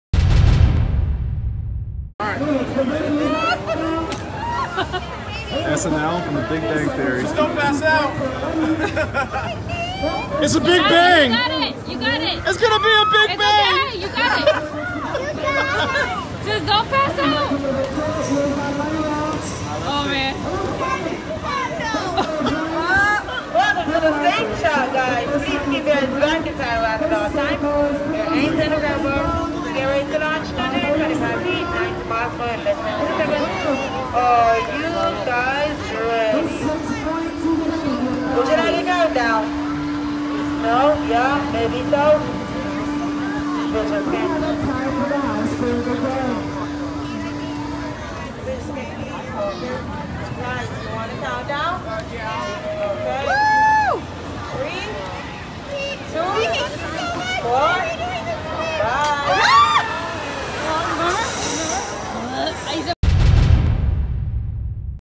Pete Davidson & Kaley Cuoco Take On-Set Palling Around to Six Flags | TMZ
Pete Davidson and Kaley Cuoco are starring in a new movie together, and it seems they're taking their on-set friendship into the real world and the latest stop is a theme park.